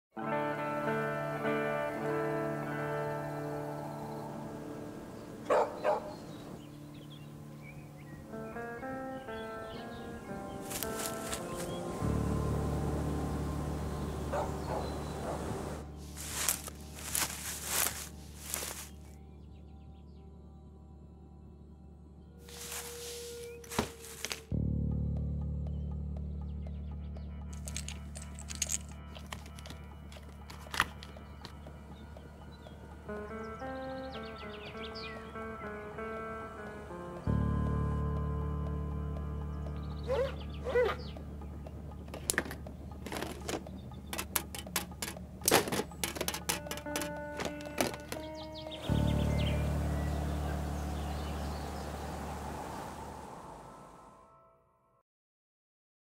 Gozdne ODREŠENI
Gozdni pobeg je igra reševanja ugank v naravi, zasnovana po vzoru escape room računalniških iger. Zberi skupino 2 do 5 soigralcev in skupaj pobegnite v gozdno sobo brez sten! Da bi prišli do cilja, morate pokazati svojo iznajdljivost in premagati vrsto čud(ež)nih gozdnih preizkušenj, zaprek, najti sledi in razvozlati vse šifre, preden se izteče čas.